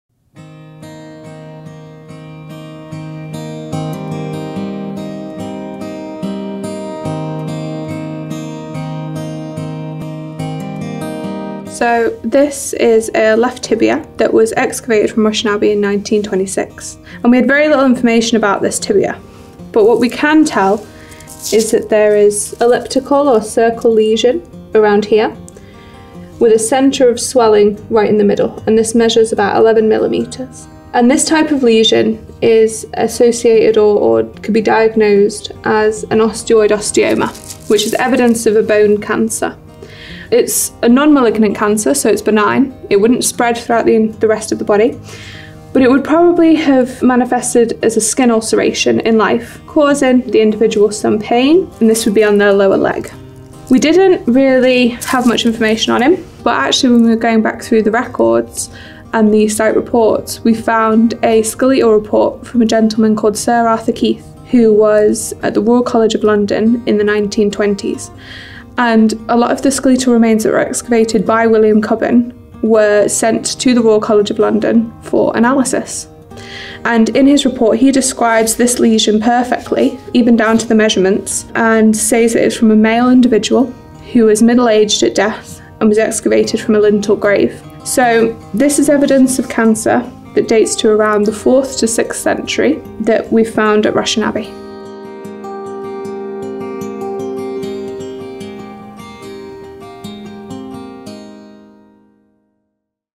Cancer in an early medieval Manx leg bone
A short film about evidence of cancer in a leg bone from c.1,000 years ago.
Dr Marie Weale presents findings relating to bones recently analysed from Rushen Abbey.

This is one of a series of films made on the bones of Rushen Abbey:

This film was made with thanks to Dr Marie Weale, Allison Fox and Manx National Heritage.
It is linked with the launch of Peter Davey’s new book, 'Rushen Abbey, Isle of Man: A hundred years of research and excavation'

Further detailed reports into the archaeology of Rushen Abbey are available

This film was released by Culture Vannin in 2024.